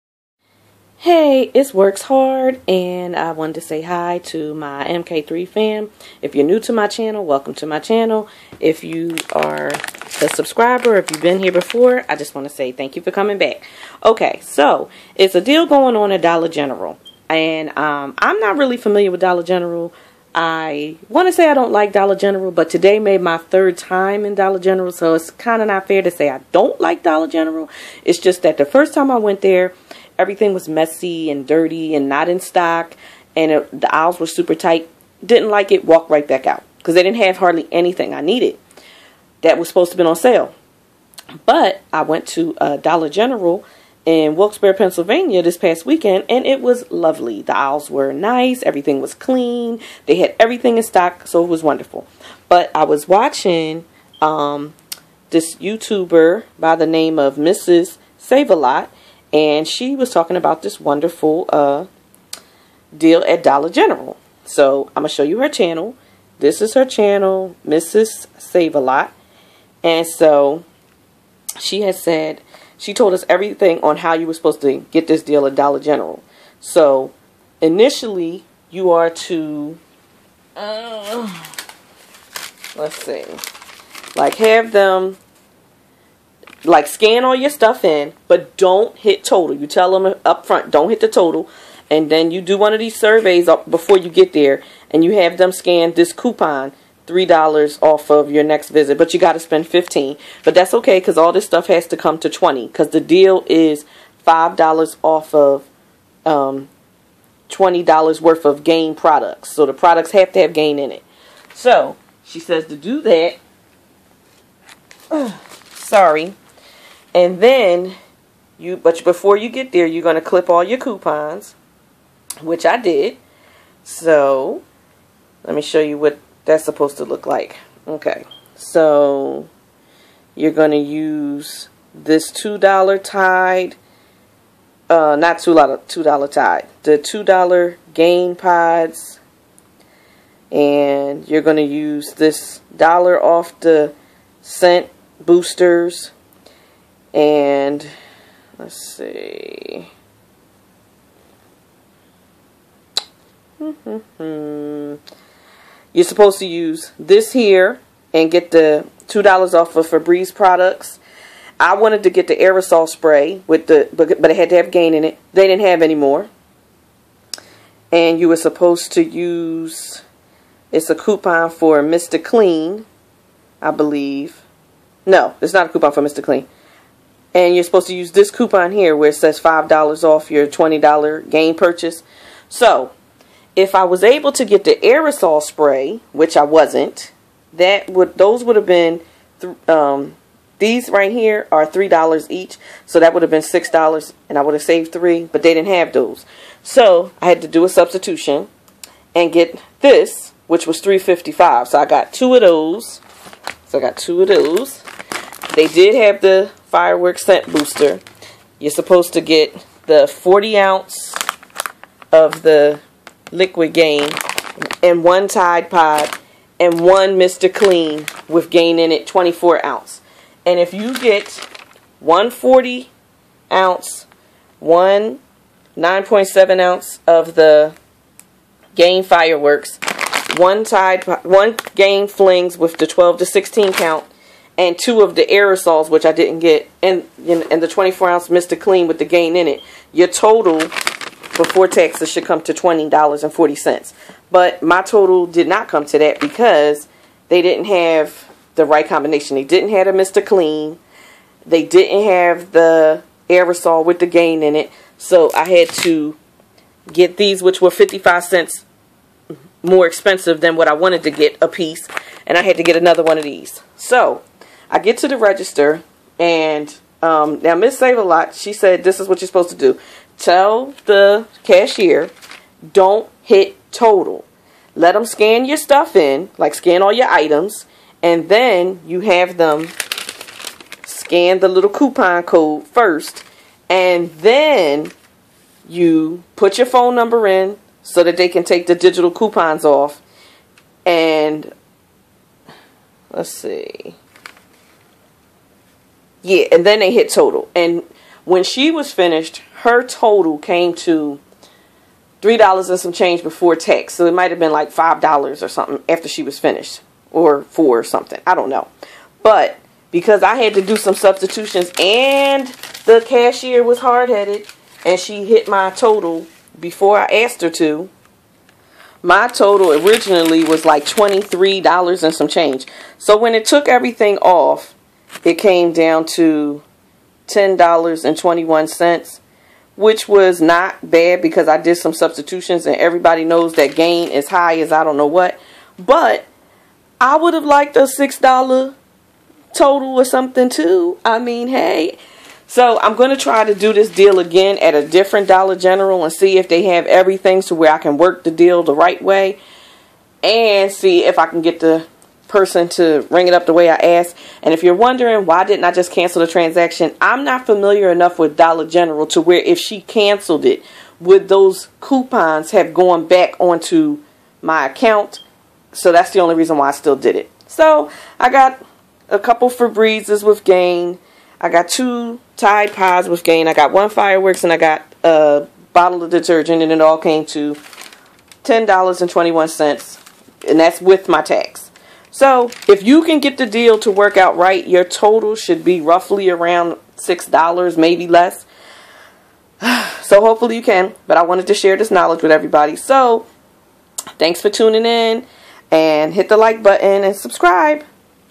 Dollar General: Couponing For Gain! Hot Deal
Check out Mrs. Save-alot's channel

Gain is expensive so if you are looking to save head to DG.